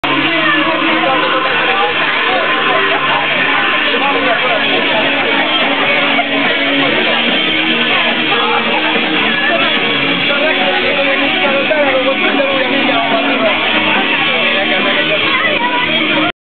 Junkies dedikálás a Headbangerben
videó a 2006. március 4-i Junkies dedikálásról. a minősége elég gyenge, de a hangulat érezhető :)
Szekeres talán épp engem és a fényképezőgépemet parodizálja.. vagy nem